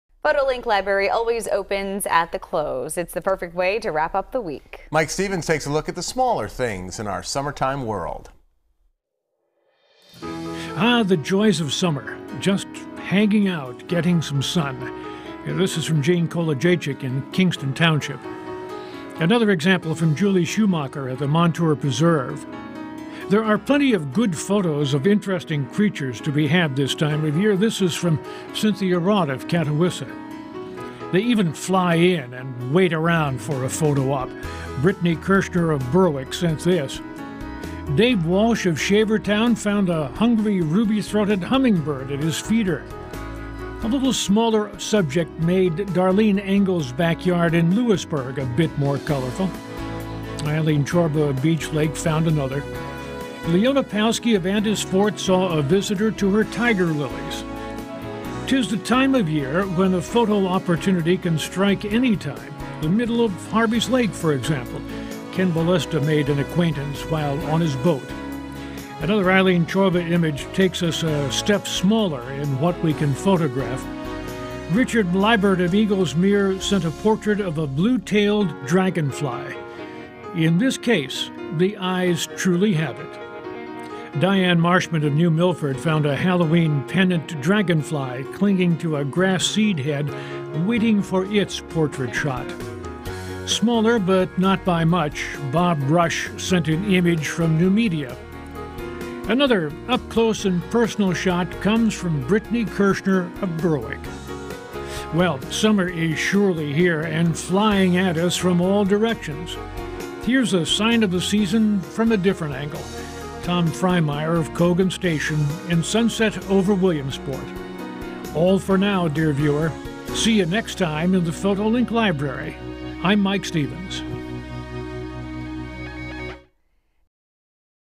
PhotoLink Library: Joys of Summer
Photolink library always opens at the close. Its the perfect way to wrap up the week. Mike Stevens takes a look at the smaller things in our summer-time world.